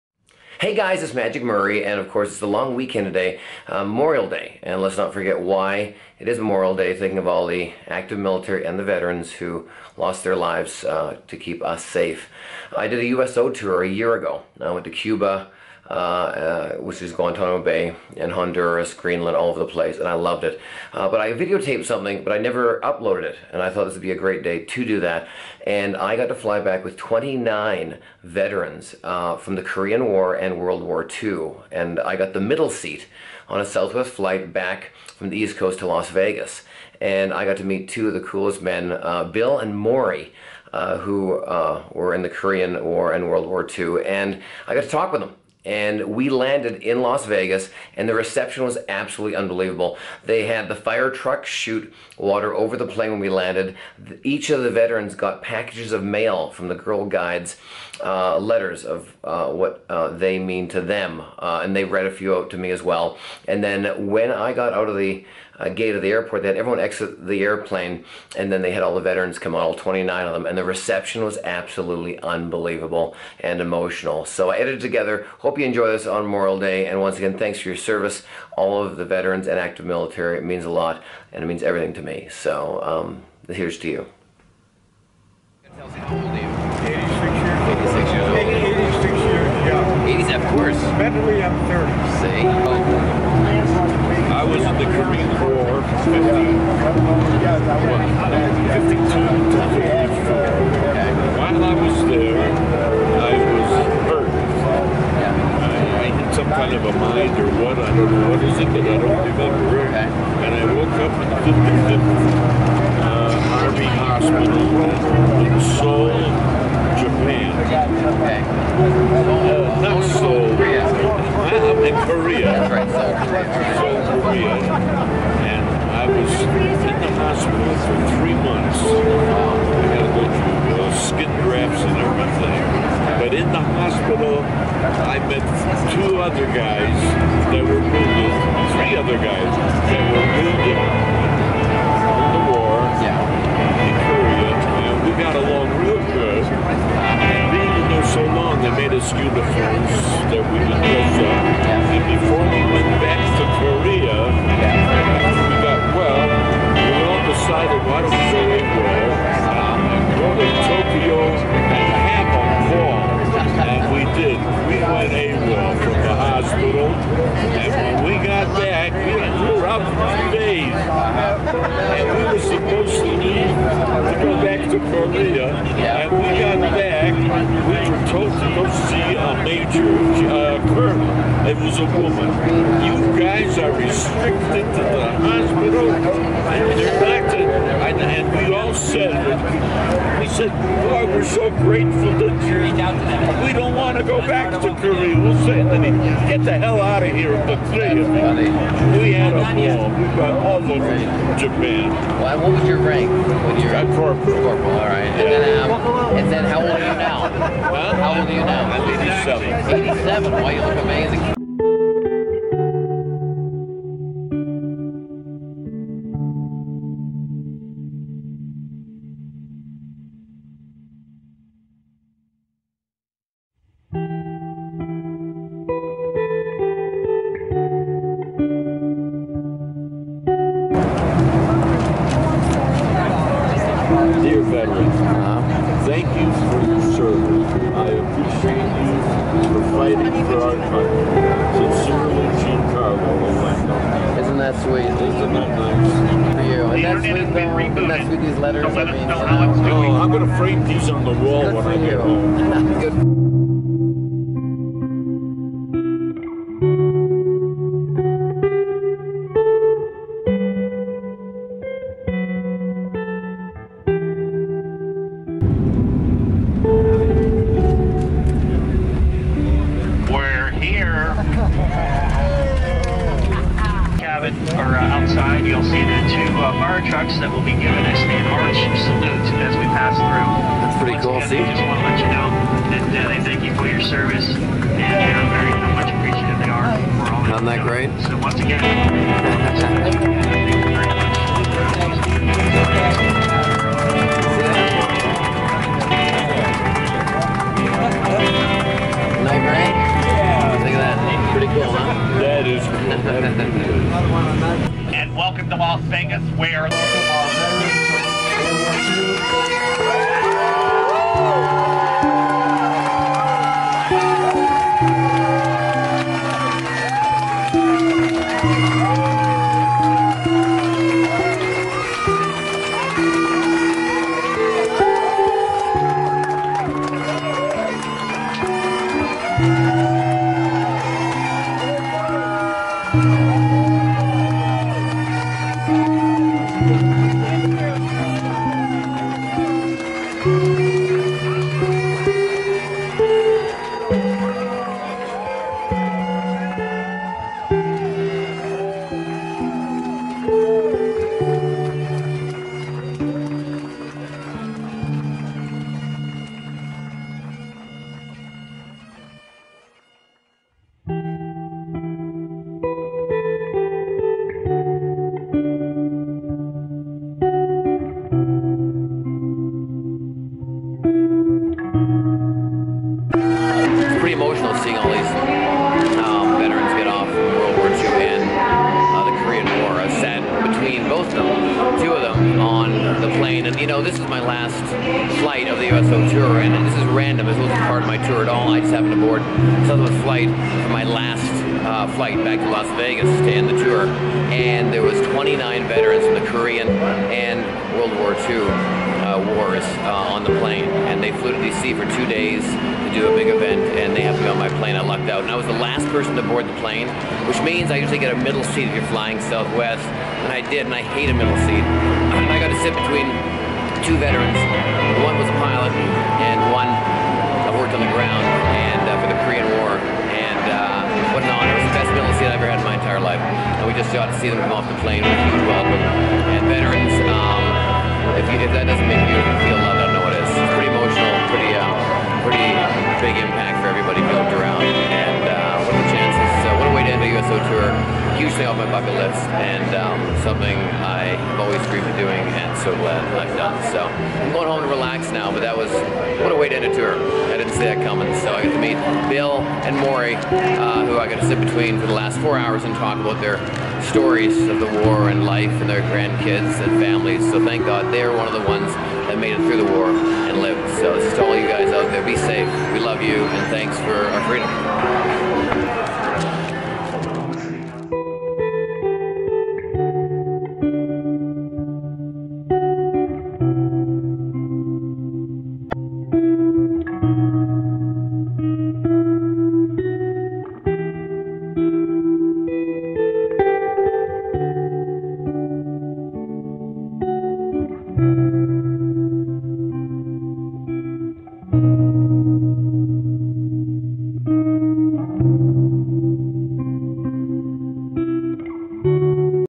SURPRISED VETERANS - World War II & Korean War Vets Surprise at Vegas Airport
Murray The Magician flies home from a USO tour and gets a surprise when he got to sit between two over 80 year old Korean War Vets named Bill & Morey. He talked with them, asked questions and what a surprise welcome they recieved at Mc Carren Airport. Thank you for all your support!